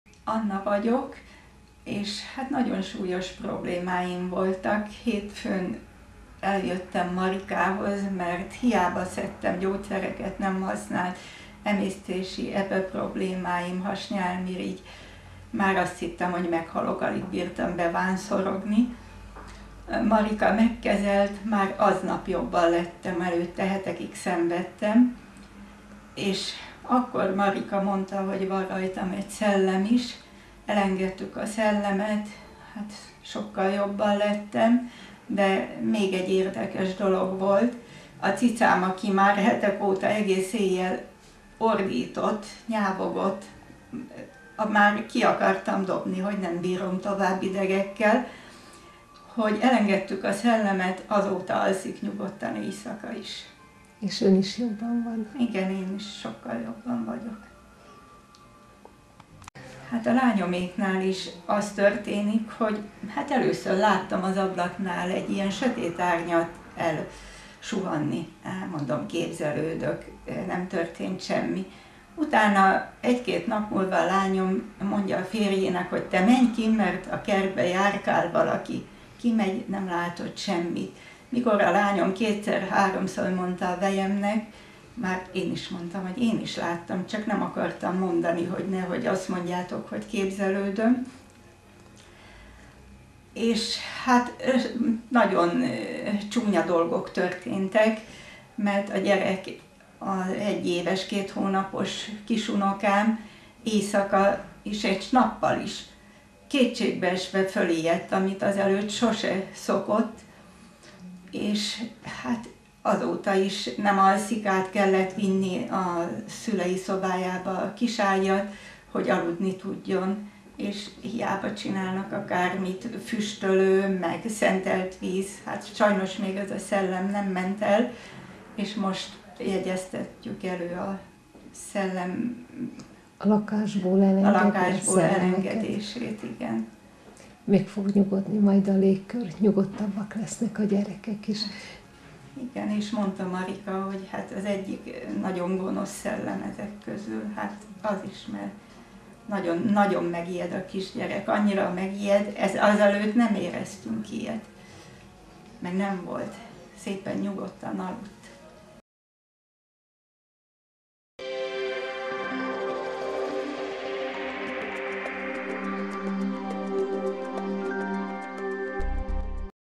Szellem okozta az unoka félelmeit
"Nagyon súlyos problémáim voltak. Emésztési gondok, epe- és hasnyálmirigy problémák kínoztak.
Eljöttem Hegedűs Máriához. Már szinte a halálomon voltam, alig bírtam bevánszorogni. Marika megkezelt, már aznap jobban lettem, előtte hetekig szenvedtem. Kiderült, hogy egy létközben rekedt szellem is van hozzám kötődve. A Táltos szertartás keretében a túlvilágra engedtük a szellemet. Ezután sokkal jobban lettem."